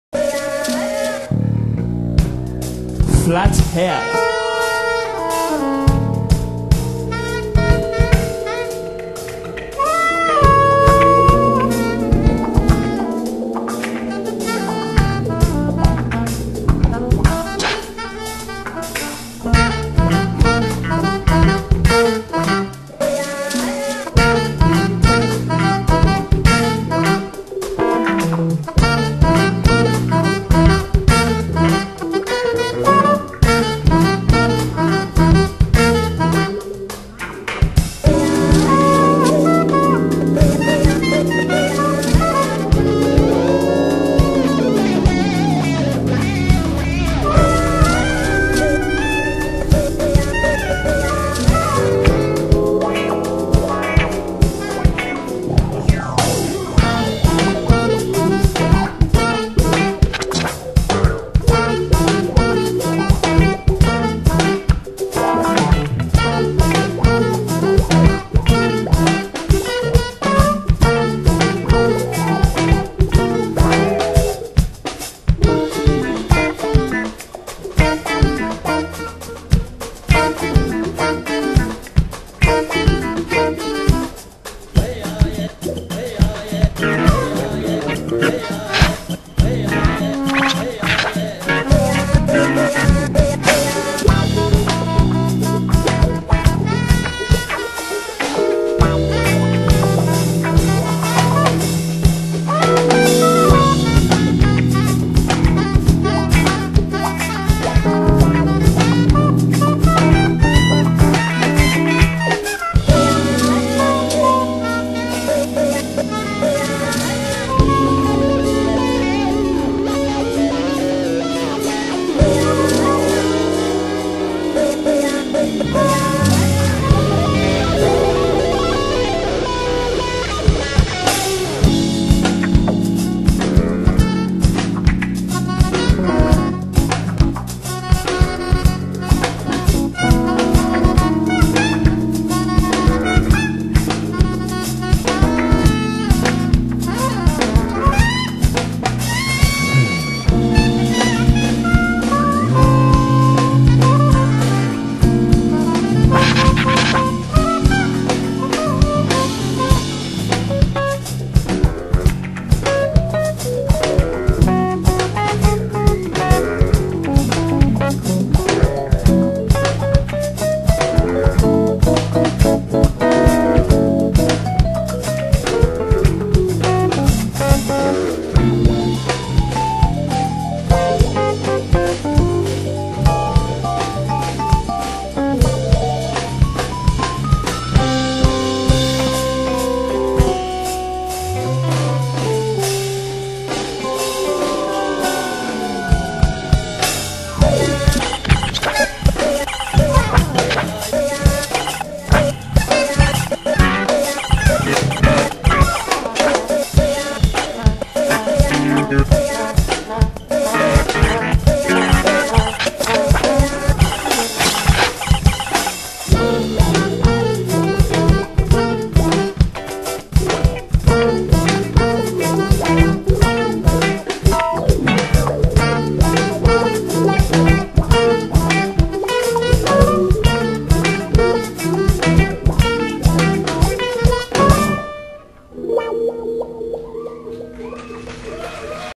FLATHEAD (FRED HAMM'S ELECTRO GROOVE BAND French FUNKADELICS
"FRED HAMM at the NICE JAZZ FESTIVAL GROOVY PARTY 2005"
This wild guitarist and his band arrive with their psychedelic funk-tinted jazz. Emulating the good vibrations of P-Funk and Bootsy Collins, their music is an unusual fusion of acoustic and electronic sounds, visiting original musical landscapes with a touch of humor borrowed from Franck Zappa. These French Funkadelics" offer a surprising and festive show in acid jazz and electro funk.